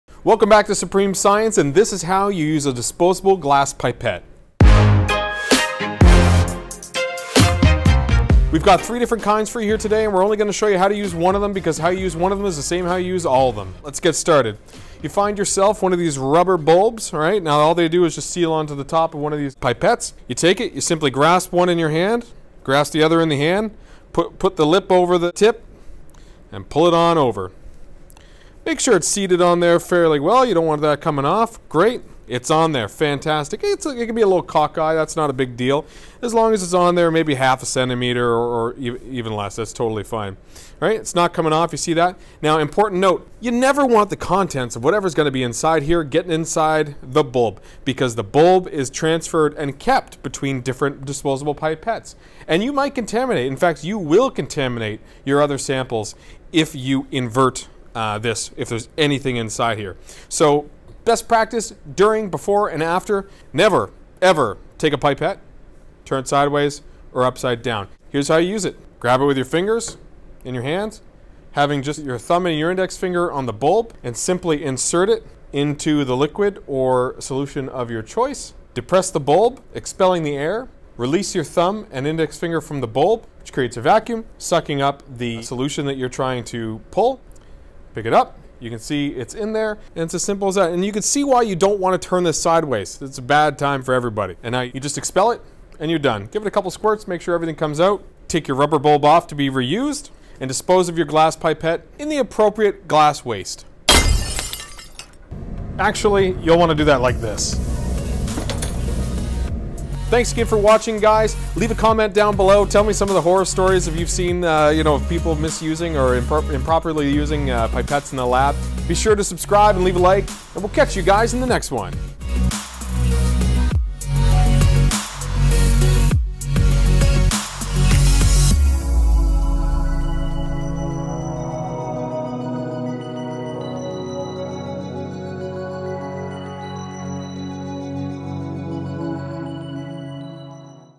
How to Use a Disposable GLASS Pipette
In this video, Travis explains how to use a disposable glass pipette